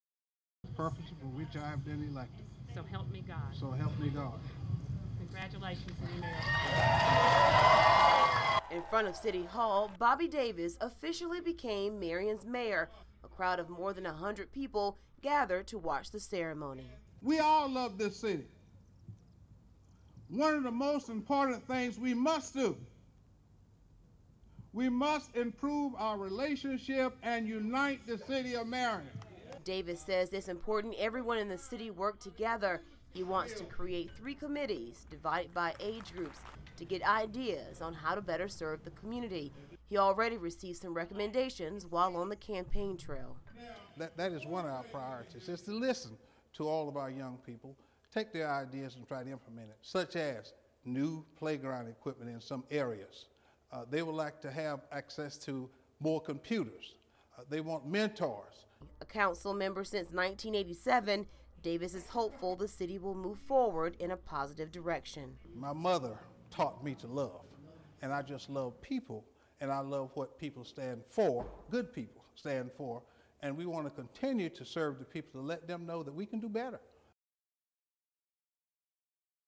Marion's first black mayor sworn in
It was a historic day for a city in the Pee Dee. Marion's first black mayor was sworn in.